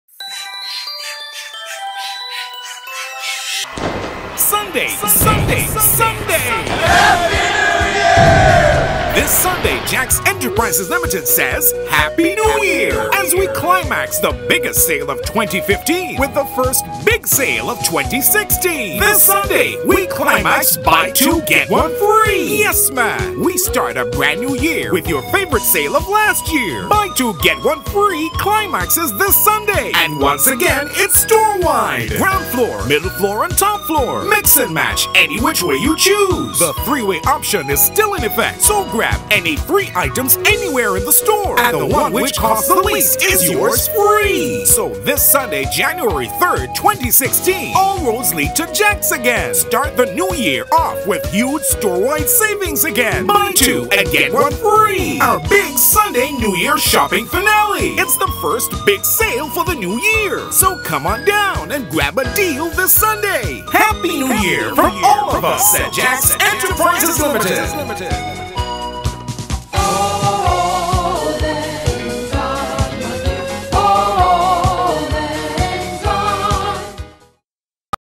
JAX BUY 2 GET 1 FREE NEW YEAR SUNDAY FINALE
SUNDAY! SUNDAY! SUNDAY!

This Sunday JAX Enterprises ltd. says HAPPY NEW YEAR as we climax the biggest sale of 2015 with the first big sale event of 2016!!!

This Sunday we climax Buy 2 Get 1 FREE!!!

Yes man!!! We start a brand New Year with your favourite sale event of last year!

Buy 2 and Get 1 FREE climaxes this Sunday!!!

And once again it’s STOREWIDE!!!

Ground floor, Middle floor, Top floor….mix and match any which way you choose!

The three-way rule is still in effect, so grab any three items anywhere in the store and the one which costs the least is yours FREE!!!

So this Sunday January 3rd 2016 all roads lead to JAX again!!!

Start the New Year off with huge storewide savings again!!!

Buy 2 and Get 1 FREE - our big Sunday New Year Shopping Finale!!!

Happy New Year from all of us at JAX Enterprises Ltd!